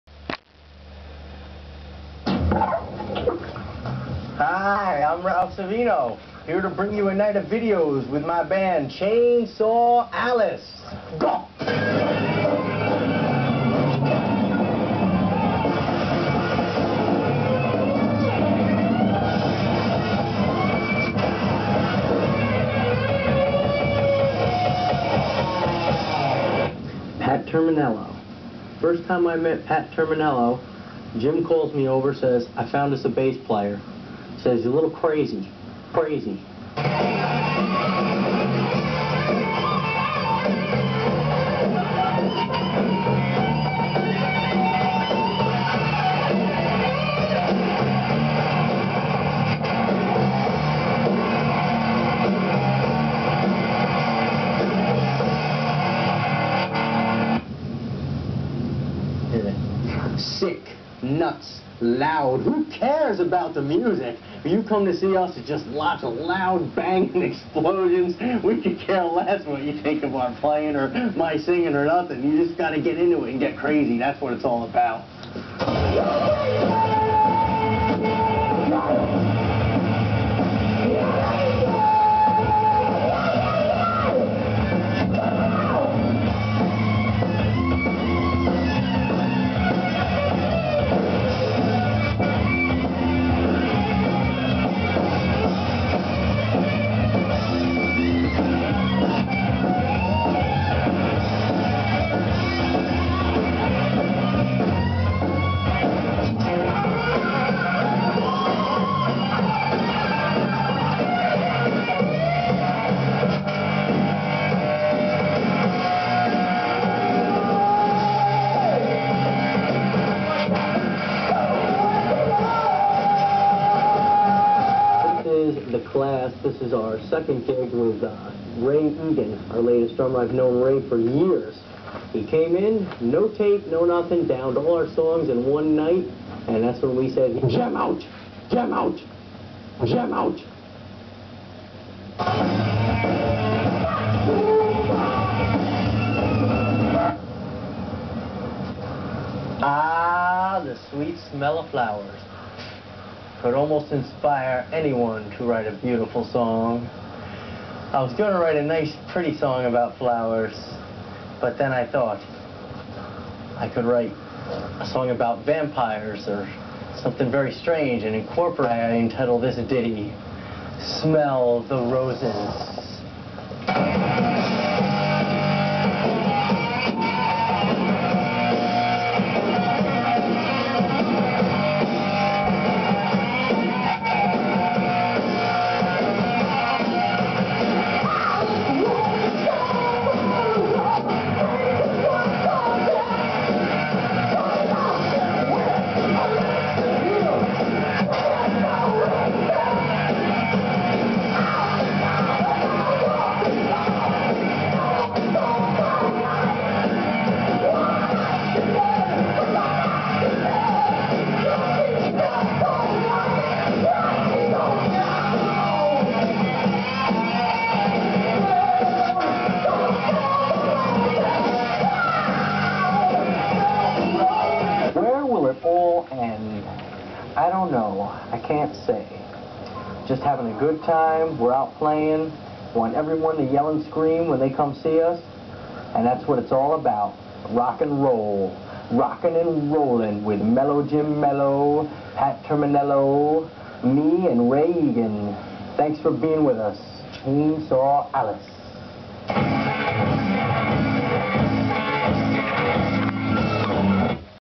Chainsaw Alice documentary trailer from 1991
"AAAHHHHHH THE SWEET SMELL OF FLOWERS. " This was a college project for a documentary on Chainsaw Alice made in 1991. This was made to be similar to and mock the MTV styles of the time with the moving camera and above and below angles. Commentary and acting by Ralph Savino, produced and filmed (documentary footage) by Jim Mello. Live Chainsaw Alice footage filmed by. well we're really not sure who shot the live footage.